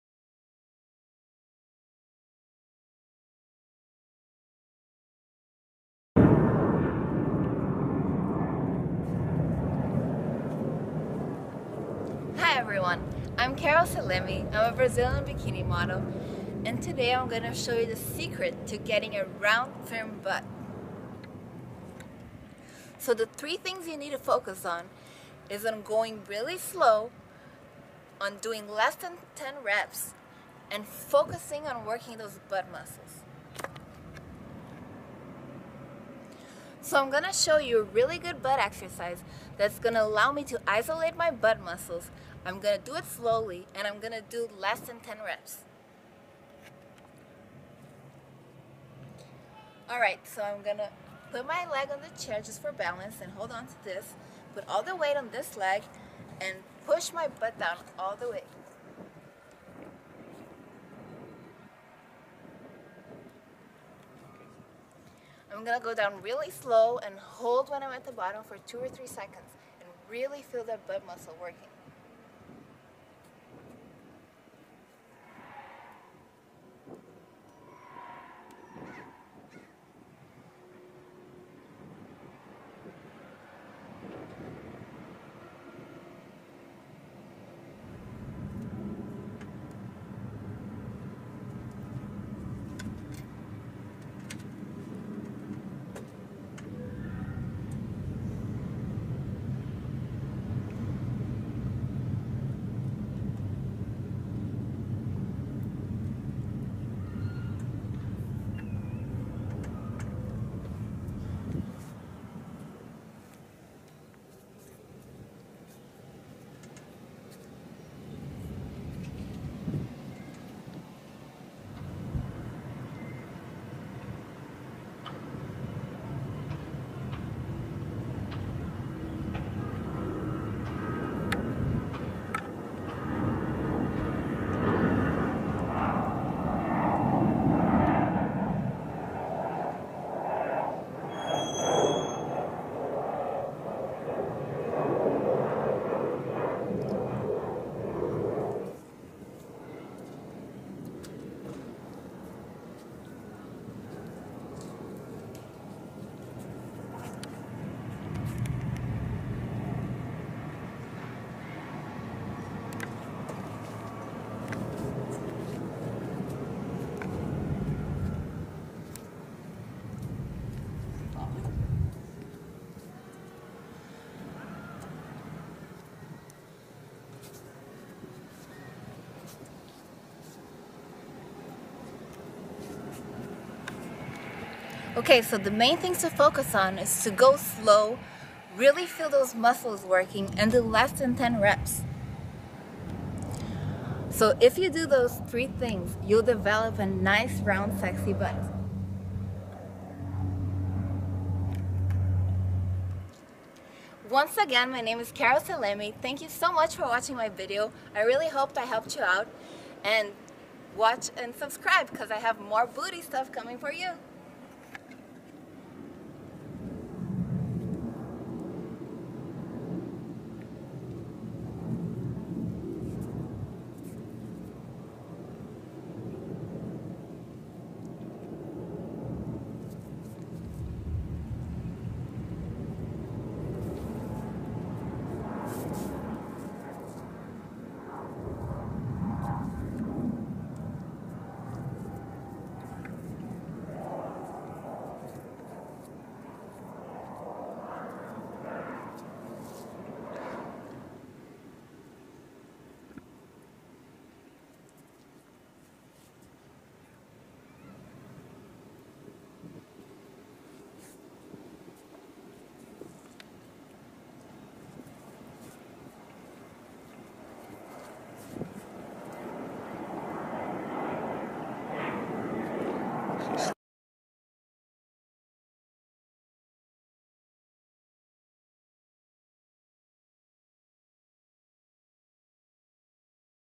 BIKINI BEACH WORKOUT
Want a sexy round Butt? Then be sure to sit it back, go slow, and really feel the butt muscles work. Train your butt at least 2-3x per week for 45min or more.
Model Carol Seleme